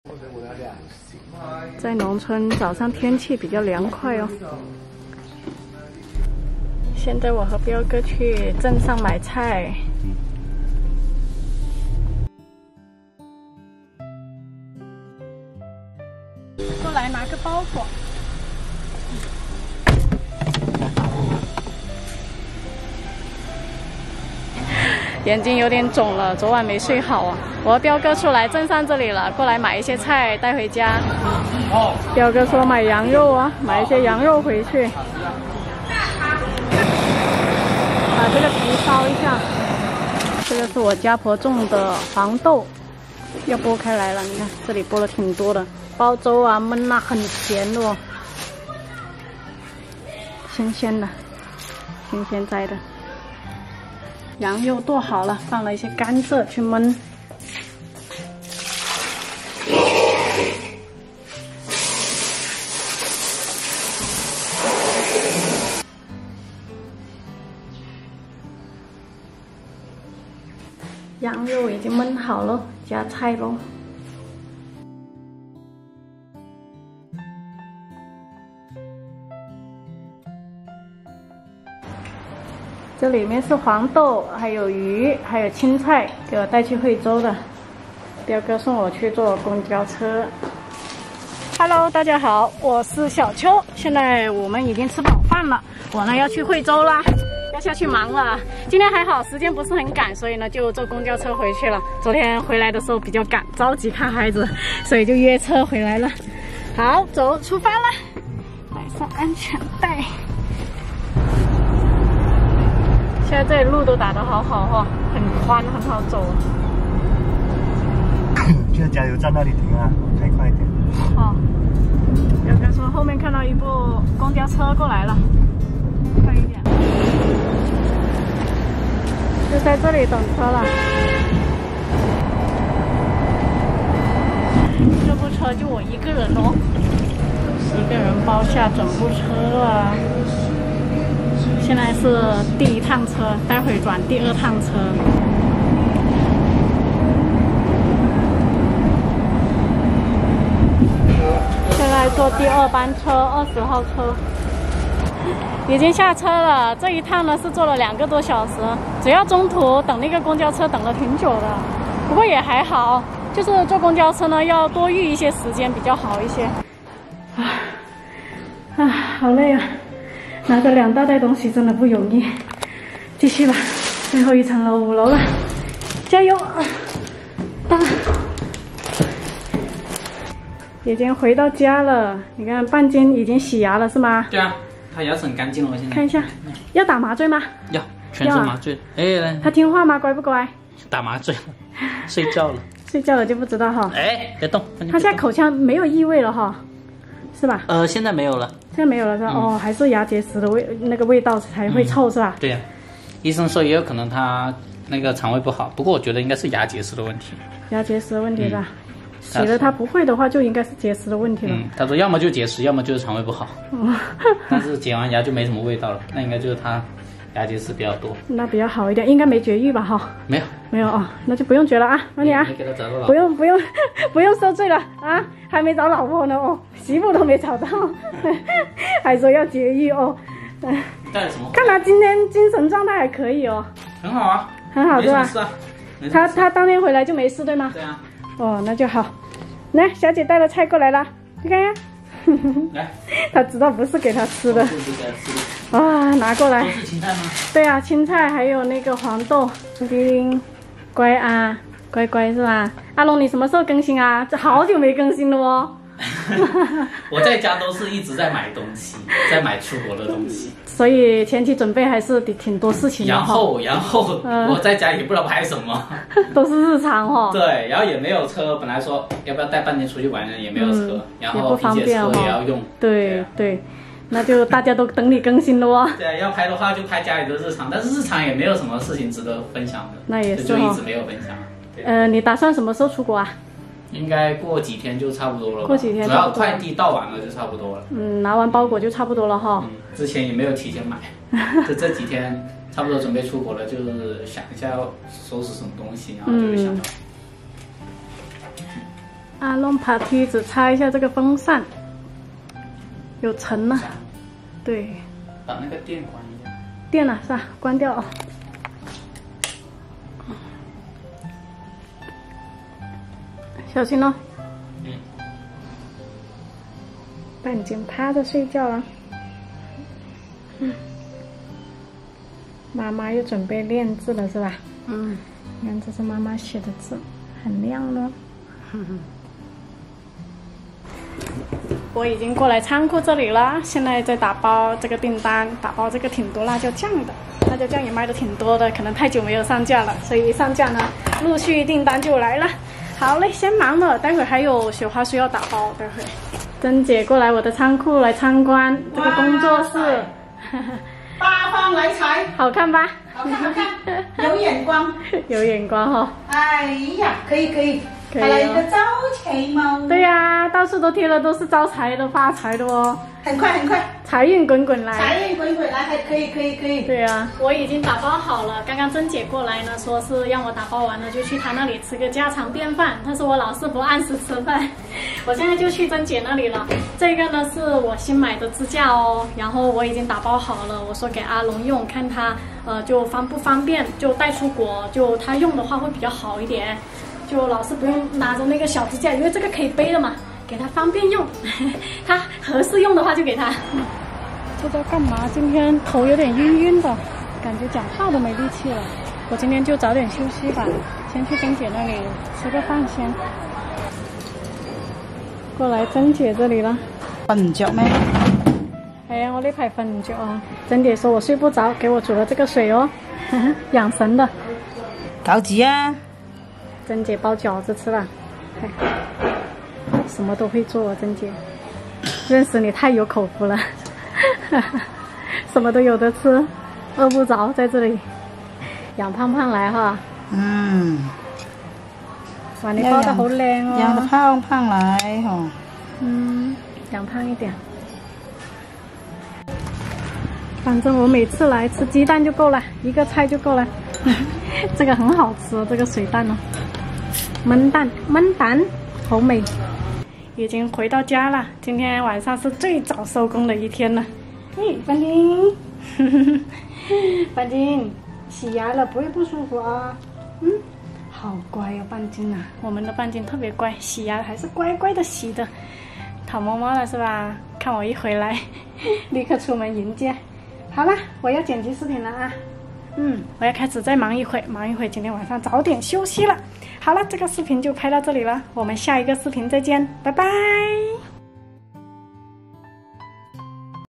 538Q：小秋真幸福公婆疼爱，半斤洗牙了，阿龙在家忙什么为什么不更新？
（因为这是阿龙的paypal，所以请注明给小秋哈）

大家好，我是阿龙的姐姐小秋，在阿龙的鼓励和支持下，我也加入了自媒体，并在网络平台和大家分享生活的点滴，如果大家喜欢的话，欢迎点点关注，感谢你们！